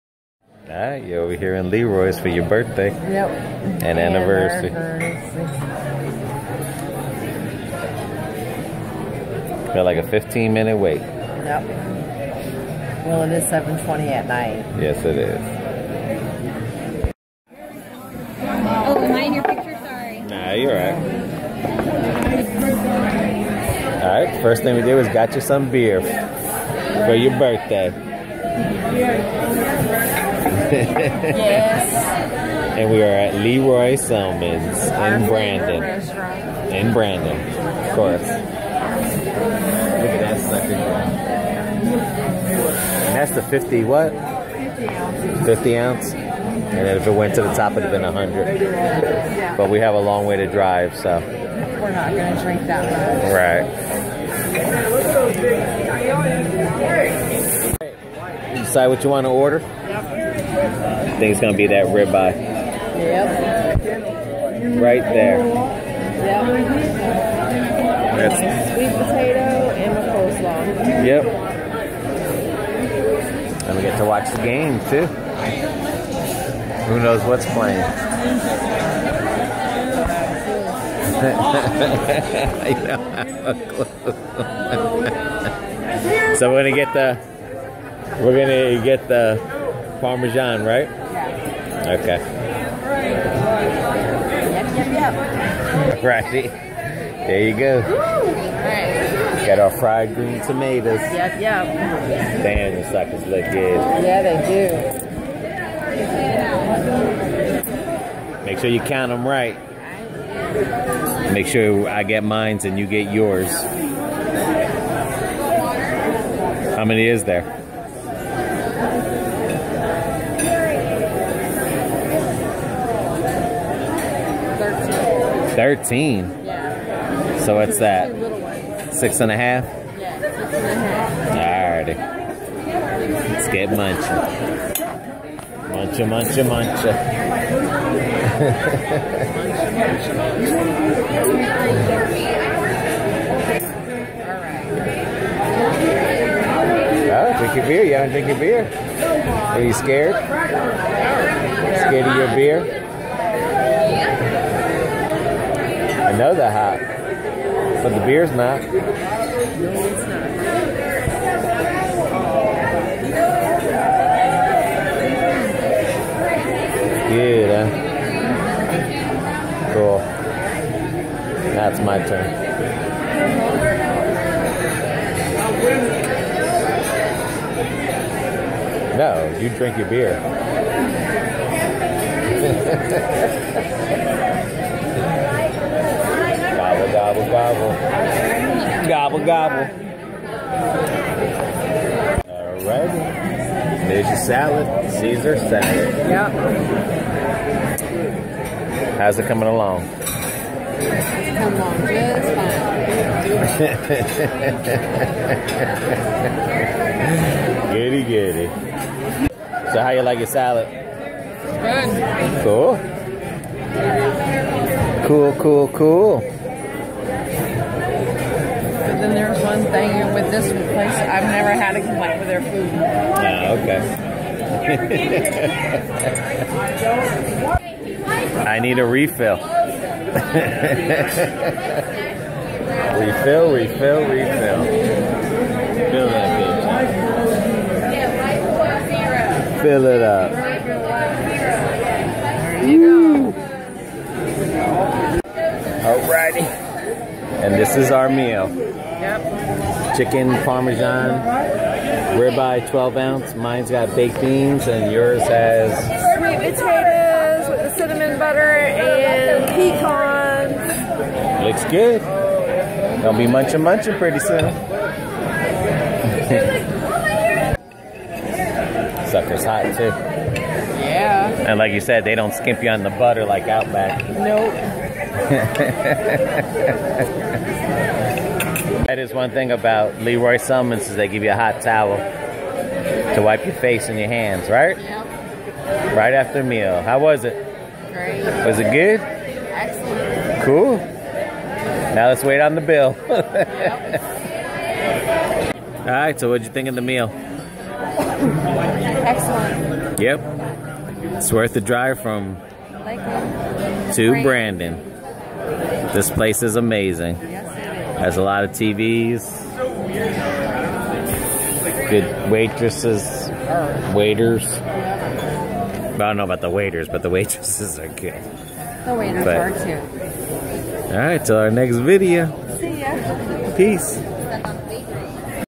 Late night dinner at Lee Roy Selmon's
I'm not a professional - I'm Just having fun - This local sports-bar chain, founded by the NFL Hall of Famer, serves Southern comfort food - Brandon Florida - 1/27/2017